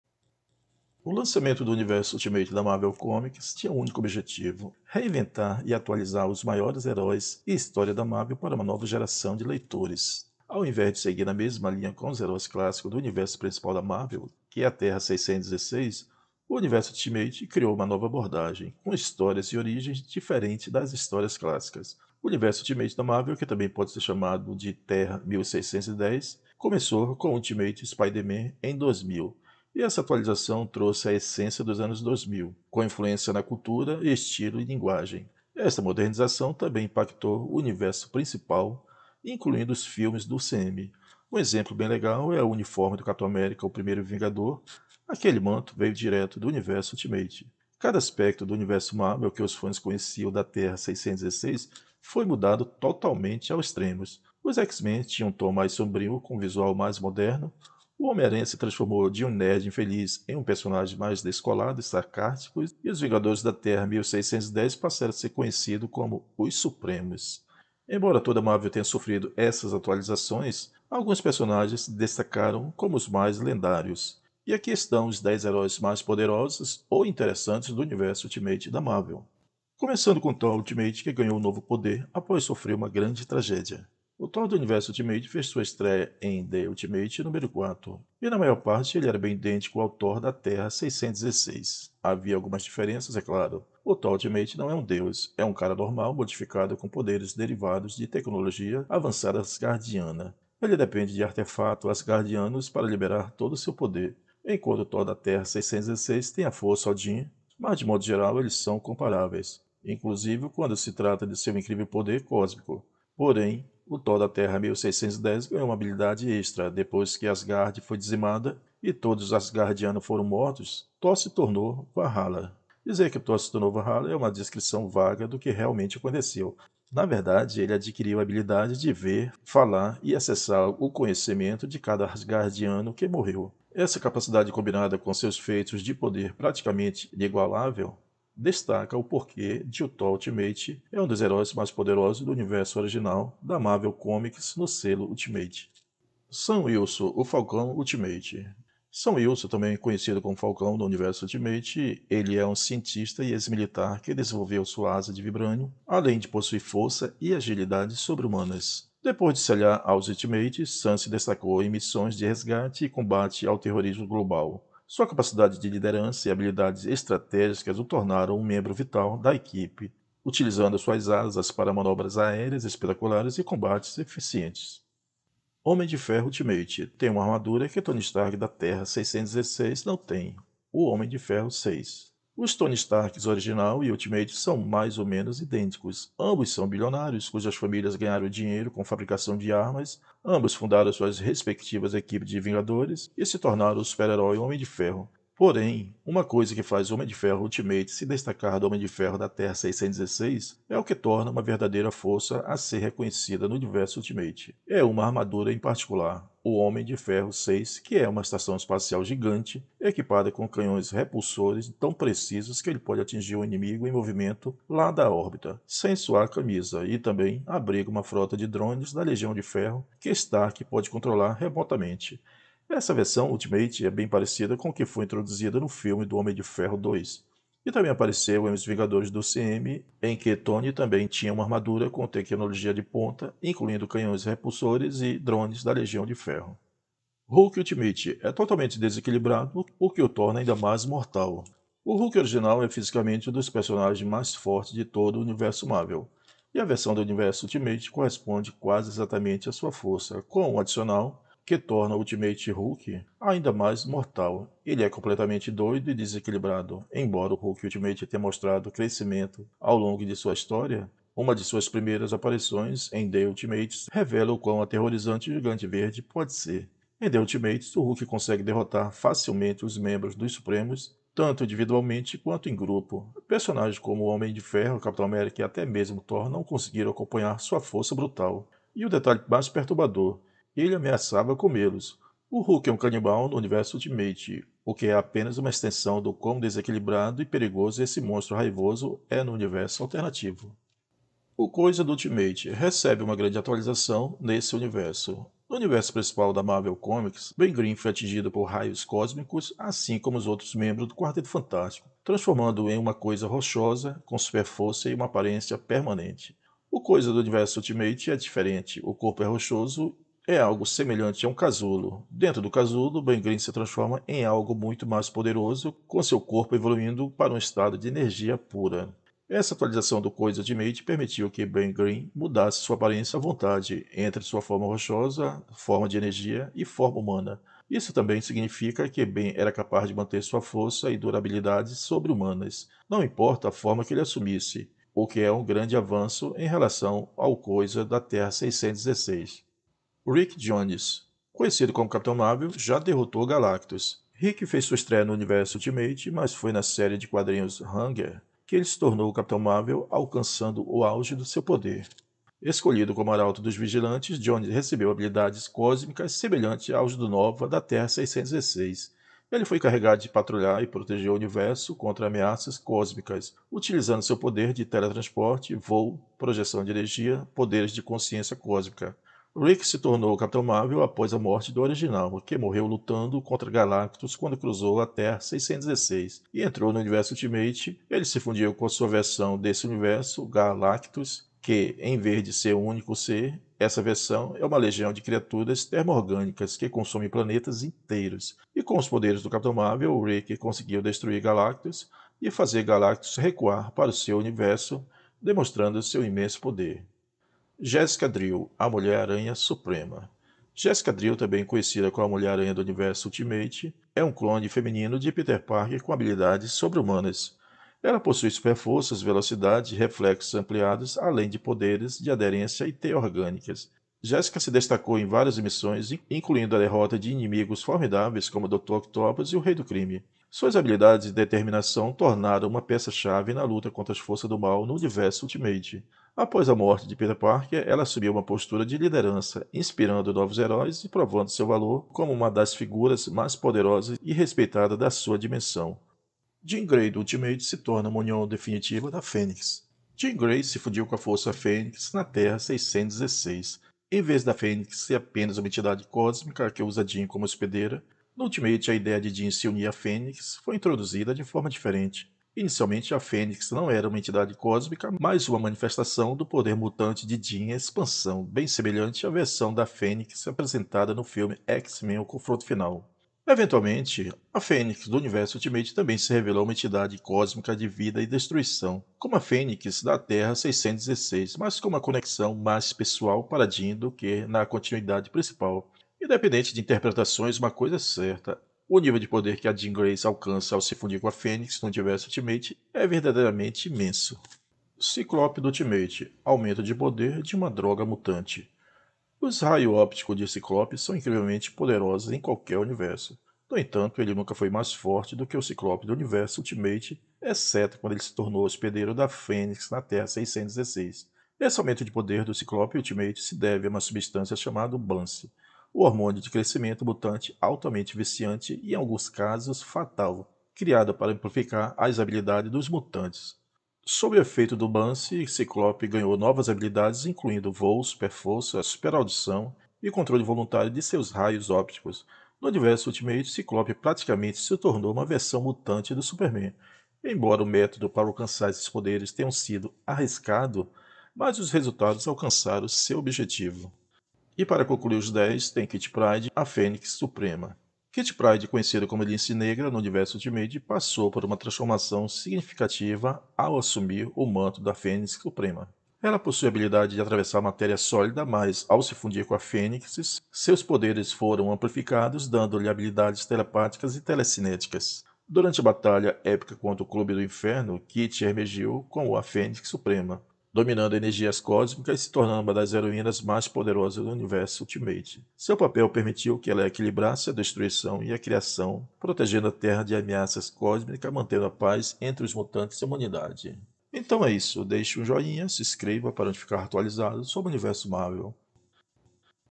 Os 10 heróis Mais Poderosos Do Universo Ultimate da Marvel
O Universo Ultimate da marvel, que também pode ser chamado de Terra-1610, e começou com "Ultimate Spider-Man" em 2000.
Embora toda a Marvel tenha sofrido essas atualizações, alguns personagens se destacaram como os mais lendários.
E aqui estão os 10 heróis mais poderosos ou interessantes do Universo Ultimate da Marvel!